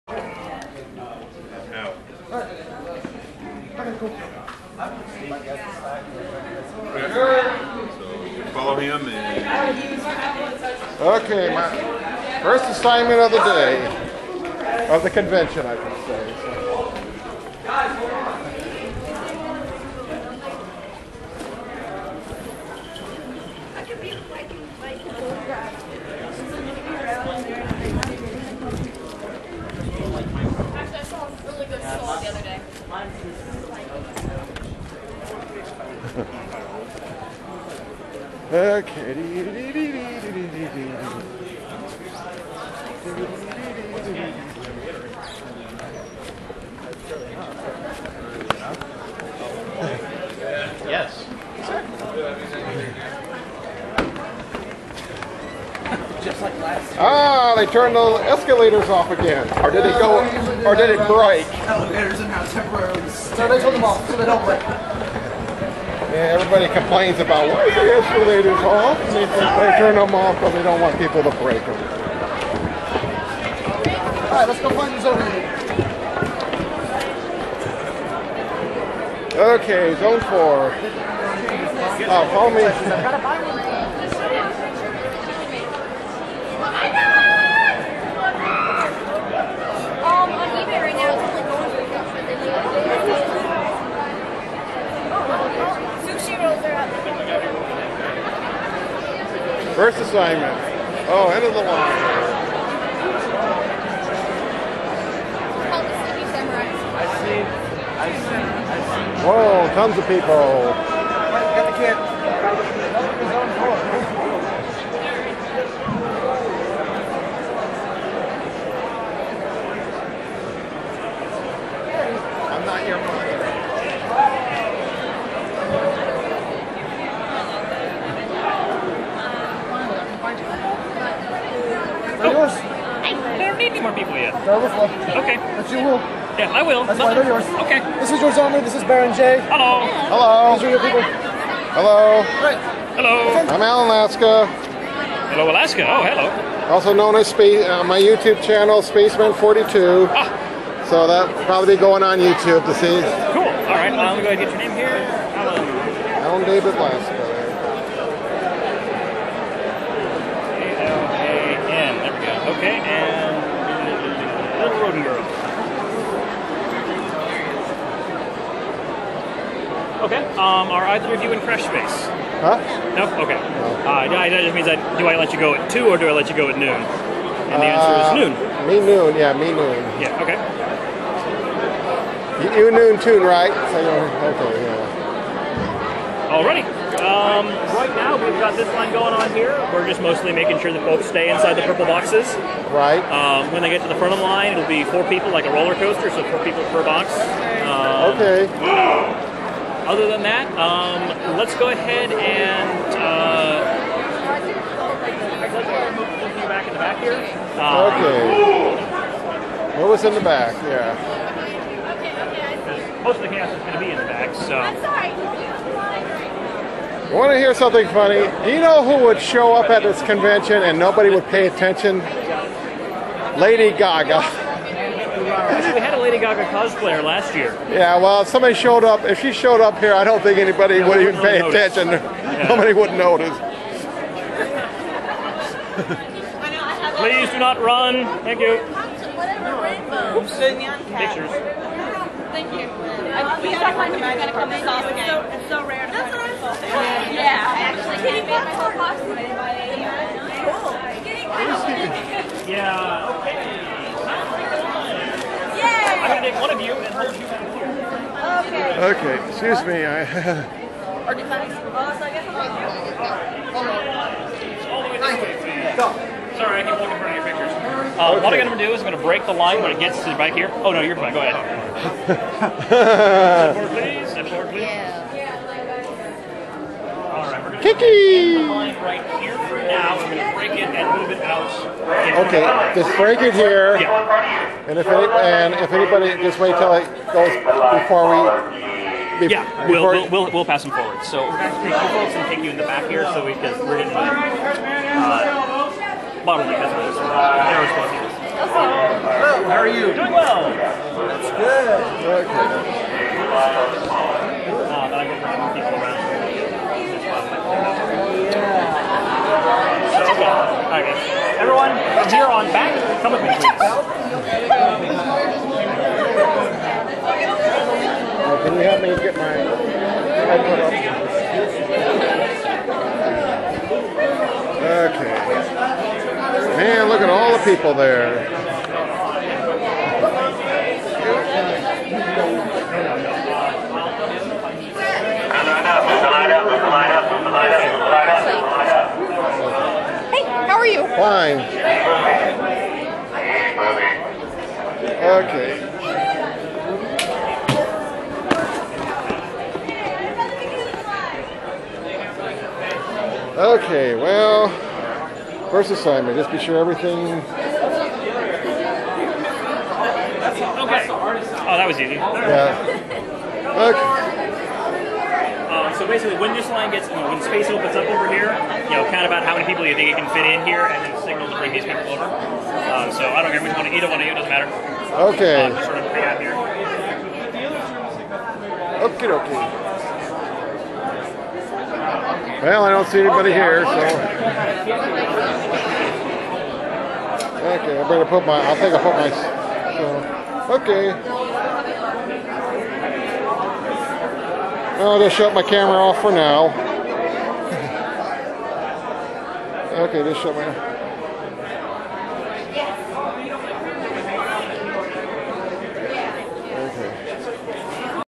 A-Kon 22 - Day 1 - Friday morning
Some small raw footage of getting my convention assignment
for A-Kon 22 of the Kon Patrol (aka convention security).

Going to keep things running smoothly at the
Pre-Registration line at A-Kon 22.

Taken Friday morning, June 10, 2011 at
about 9:00am CST.
At the Sheraton Hotel Convention Center in
downtown Dallas, Texas.

INTERESTING NOTE:
---Due to some Dallas city ordinance anyone that uses
the term security as convention security, can not use this
term.. It is considered that you are a professional security
personnel. That is why there is nothing that is
called A-Kon Security---